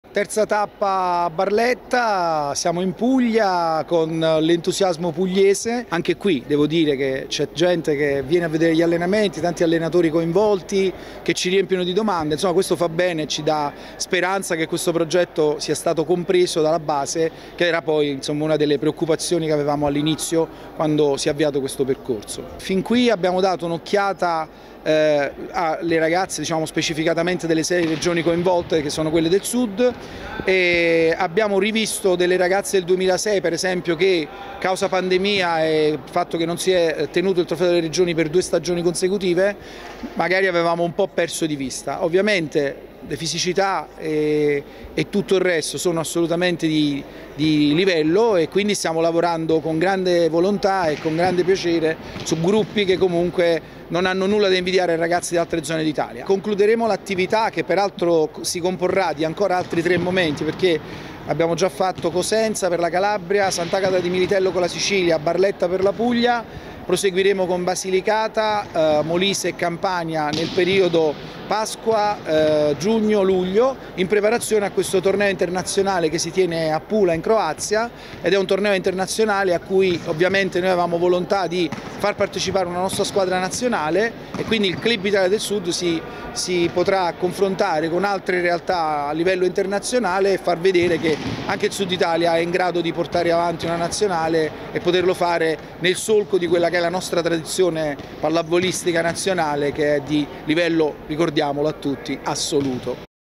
LA FIPAV A CACCIA DELLE AZZURRE DEL FUTURO
Si è conclusa a Barletta la terza tappa del progetto "Club Italia del Sud" della Federazione Italiana Pallavolo. L'obiettivo creare la "cantera" delle giovanissime pallavoliste del futuro partendo dal Mezzogiorno d'Italia.